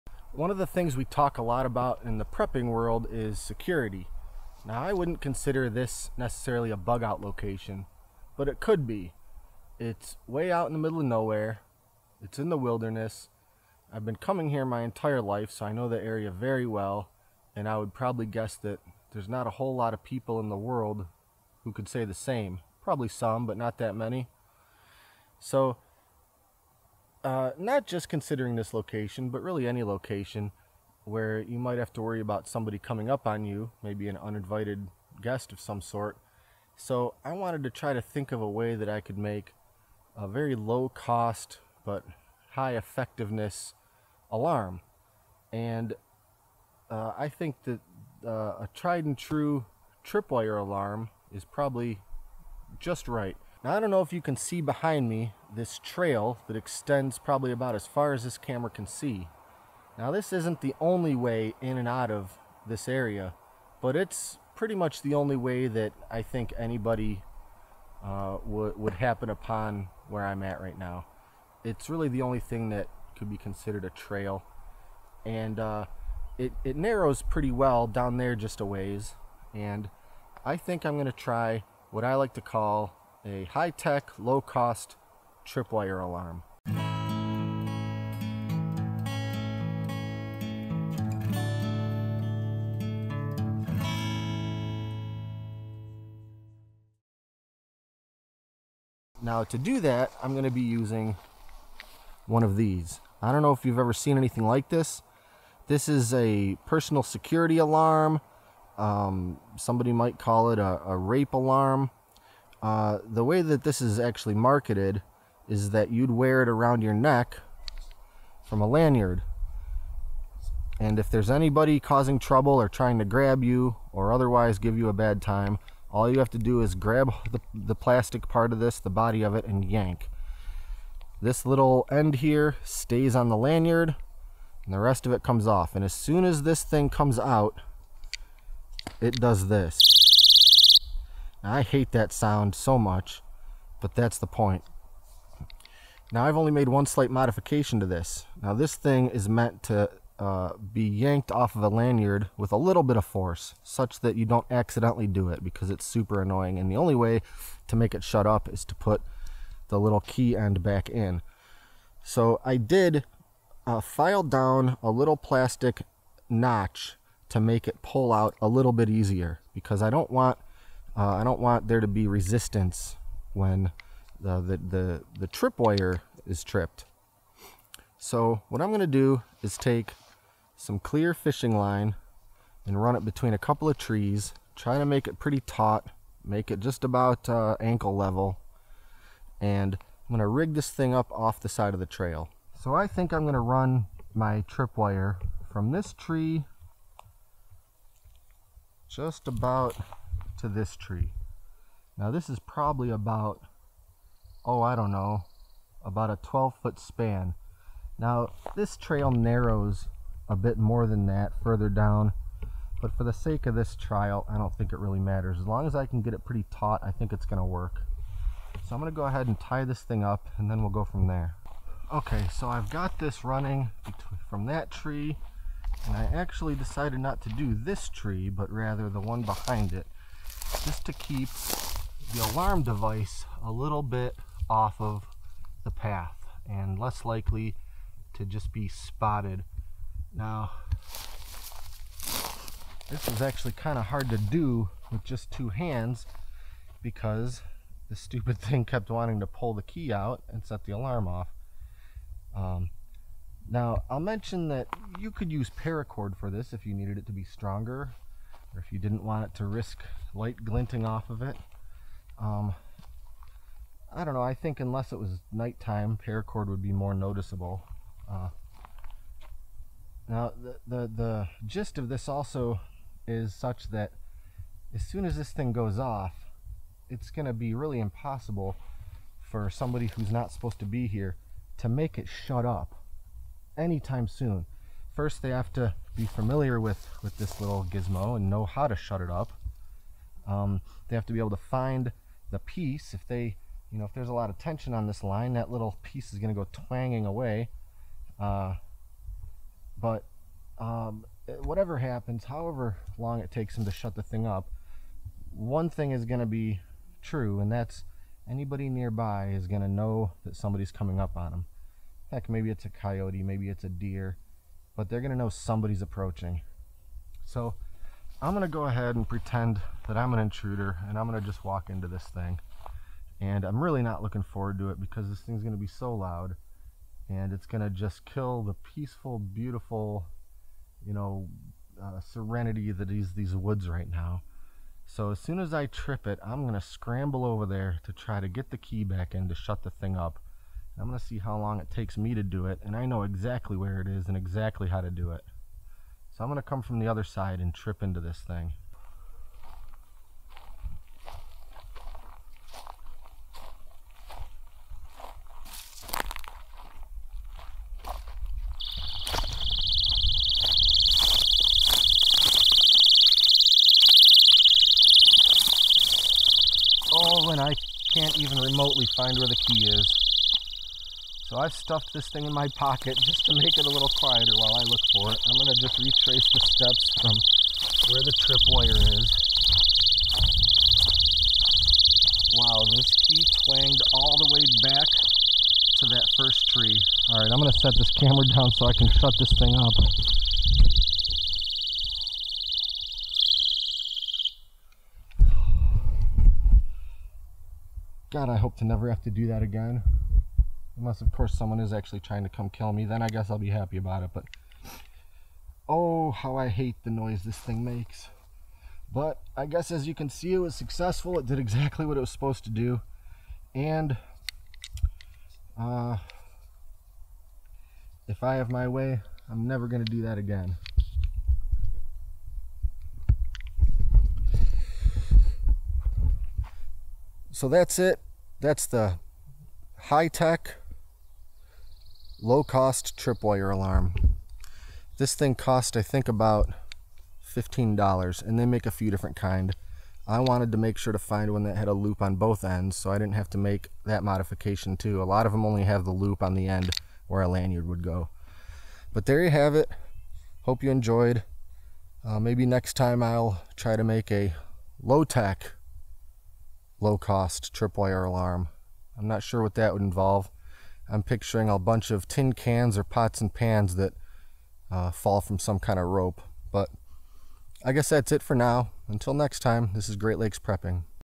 Hi-Tech / Low-Cost Electronic Tripwire Alarm
I had this idea a couple years ago for a very loud, very simple electronic tripwire alarm. It doesn't use radio signals, wifi, or anything like that. It's just a simple tripwire that makes a heck of a lot of noise if somebody triggers it. At a cost of around $15.00 plus some fishing line or cordage, you can be sure to know if somebody wanders their way into your area unexpectedly.

Here is the electronic alarm I used: